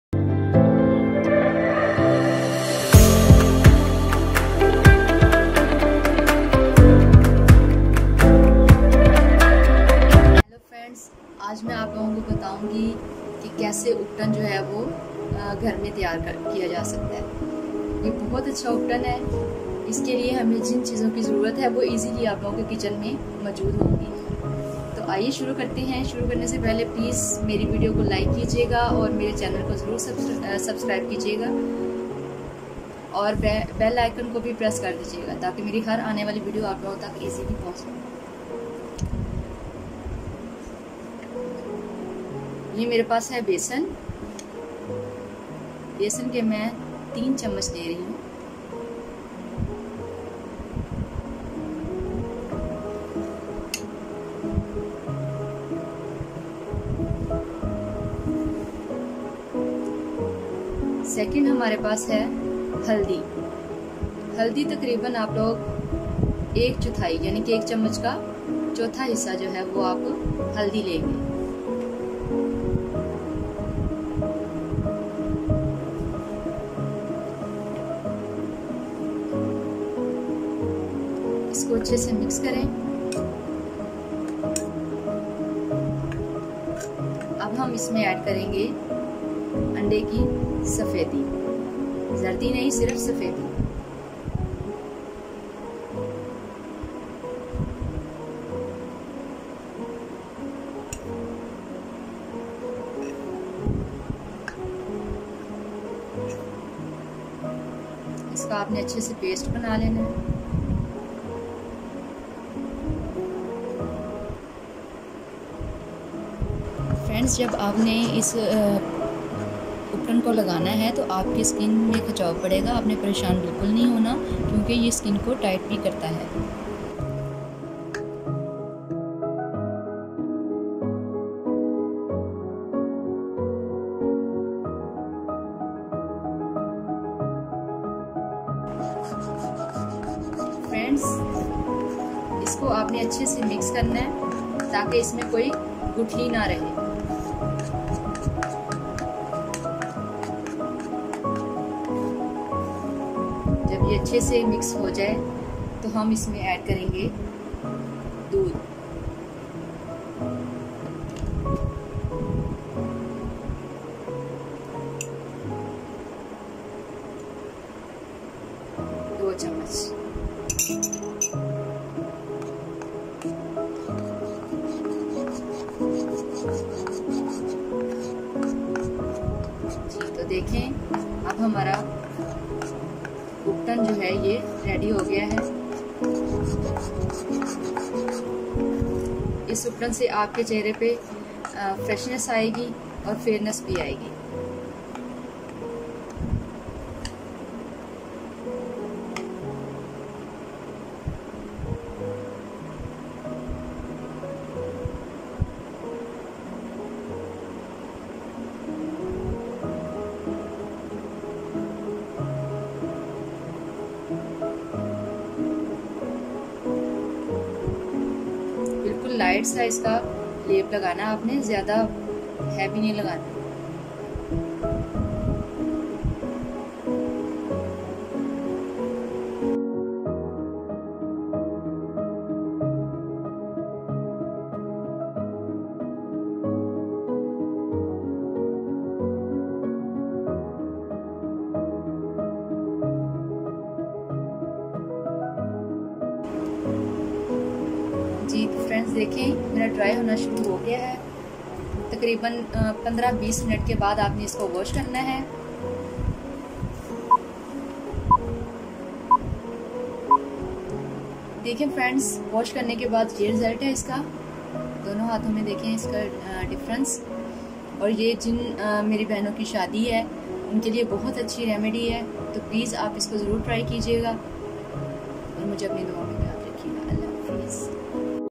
Best Ubtan Cream |Mehndi Ubtan |Finebeauty
aaj main aap logo ko Ubtan  banane ka tarika btane wali Hoon  is Upton ka bahut hi zabardast result hai .
Fine Beauty